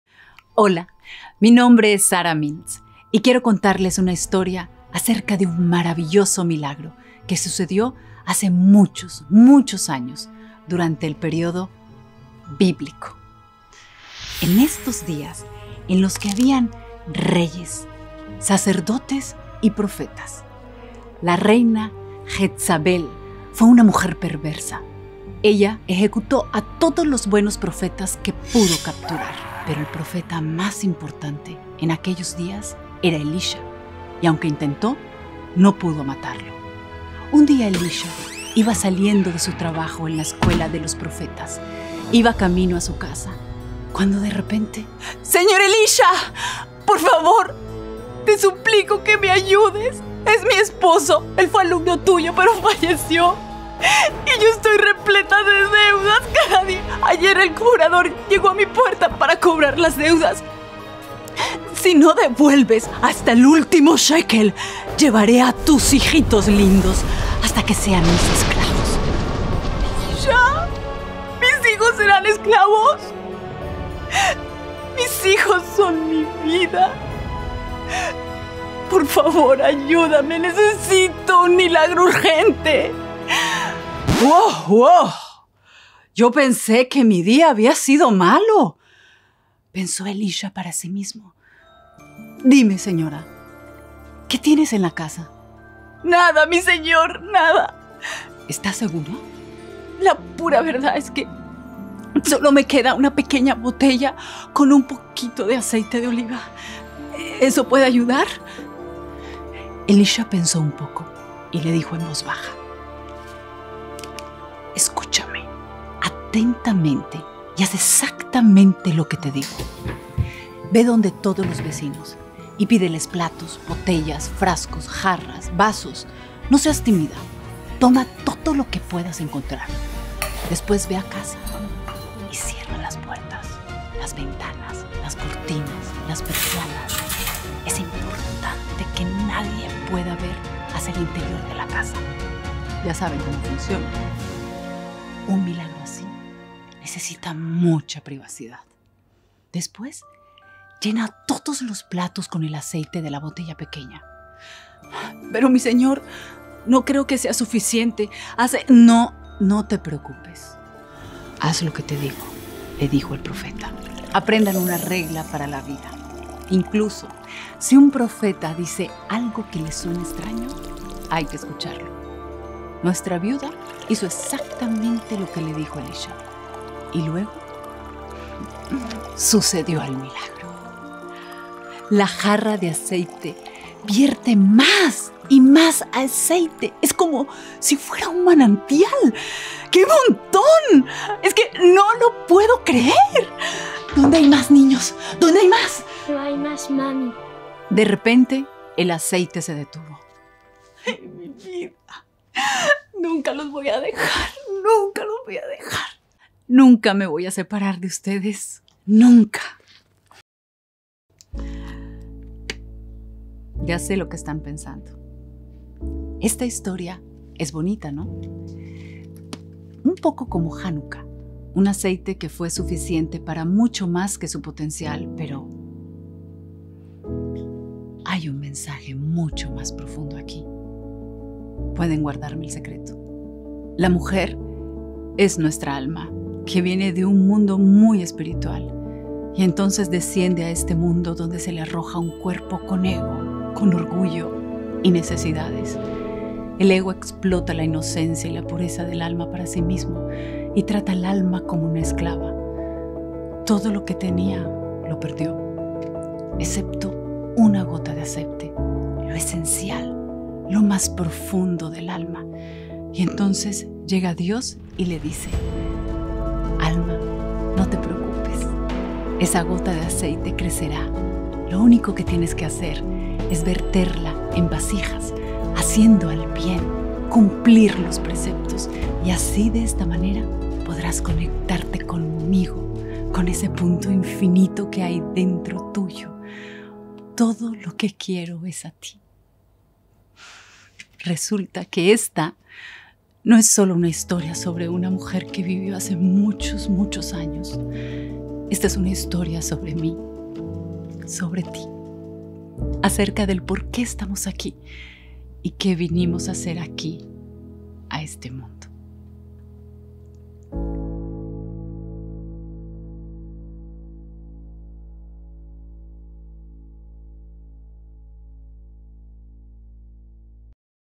Leyendas judías: el milagro del aceite
Había una vez una viuda que se quedó sola criando a sus hijos después de la muerte de su marido.
¿Pero cuáles son los medios por los cuales se pueden criar a sus hijos?
Resulta que un frasco pequeño y una gran promesa pueden cambiar vidas
¿Qué opinas de esta historia? te leemos en los comentarios